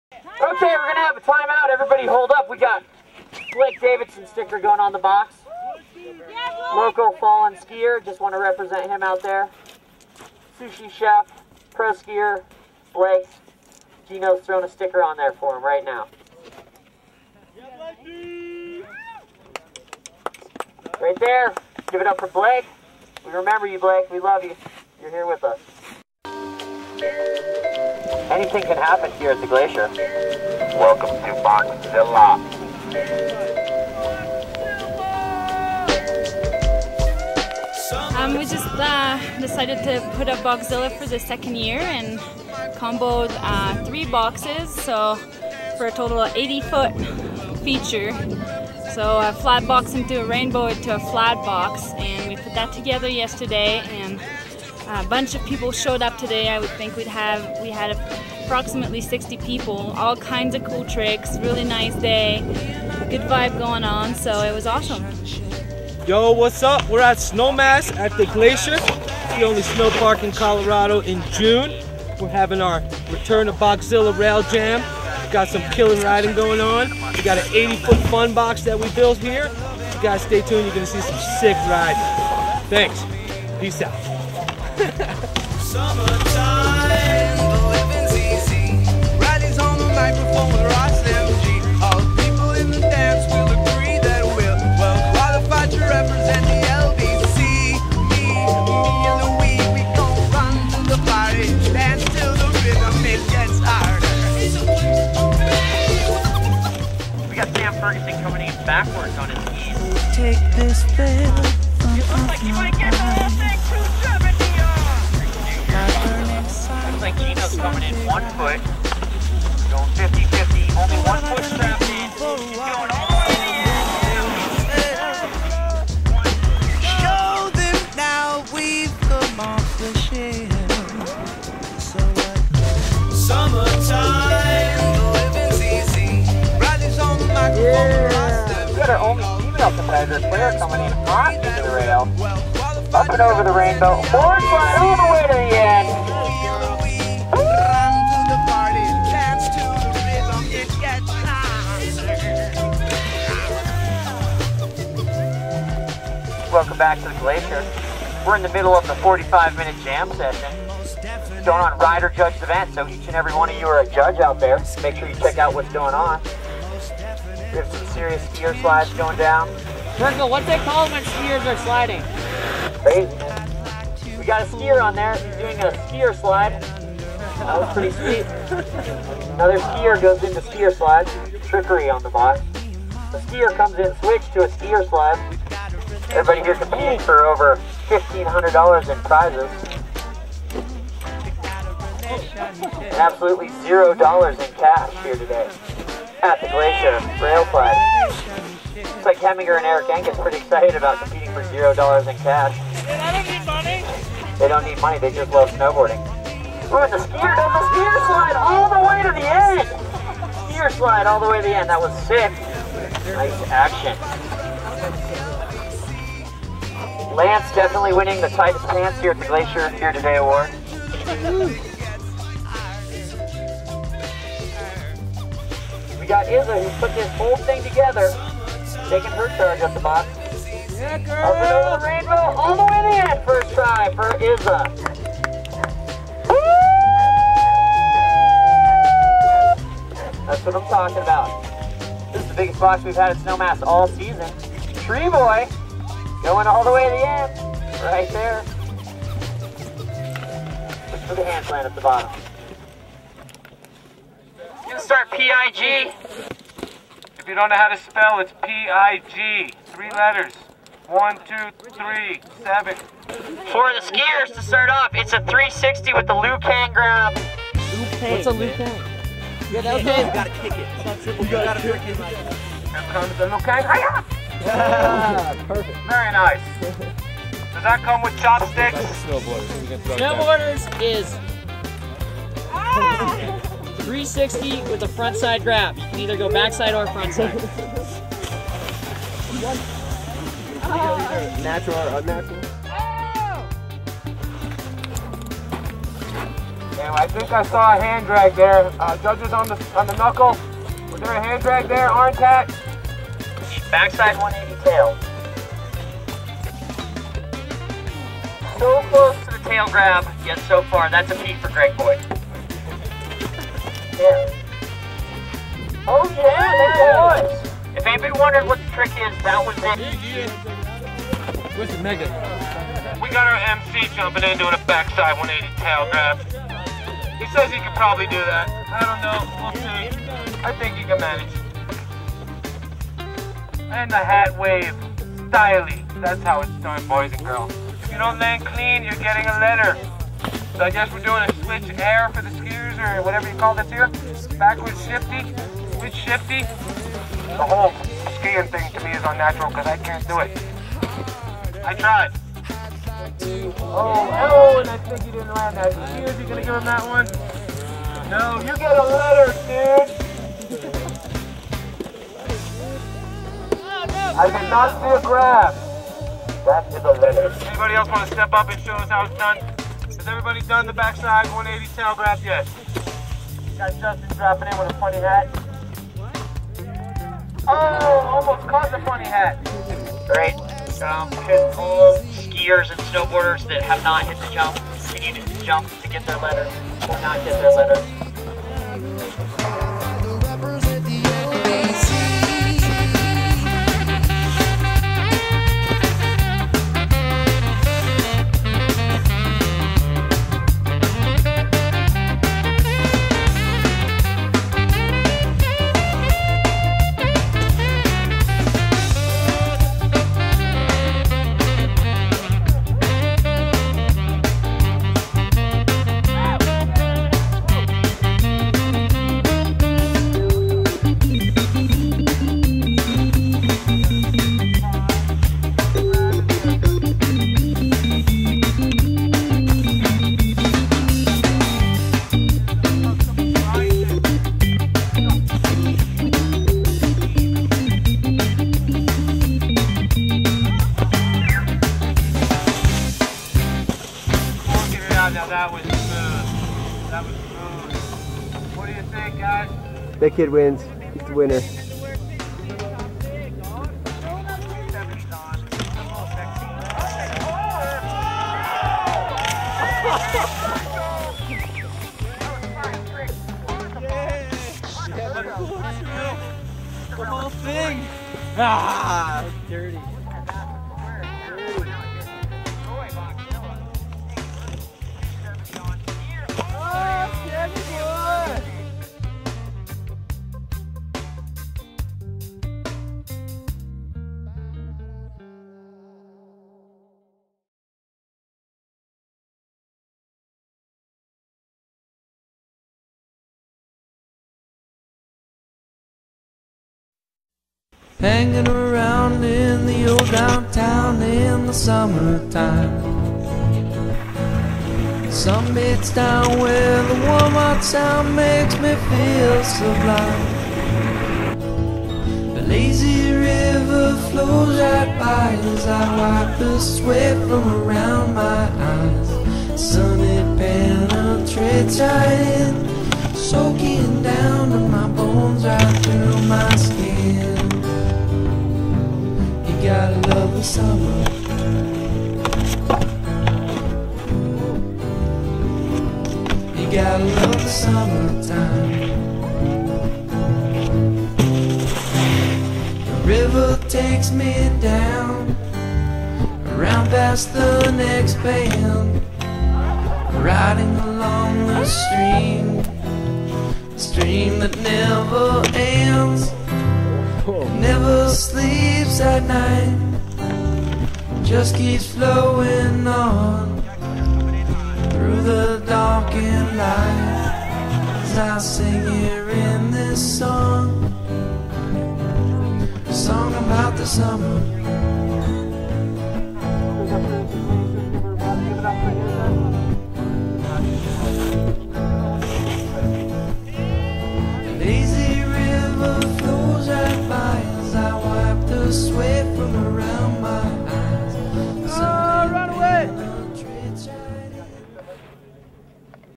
Glacier Games
Coverage of the June 23, 2007 Glacier Games at Snowmass. A jam session on 80-ft. "BOXZILLA" and a game of P.I.G. on the biggest jump...ever! Starring many skiers and snowboarders from the Roaring Fork Valley and dedicated to the memory of fallen skier Blake Davidson.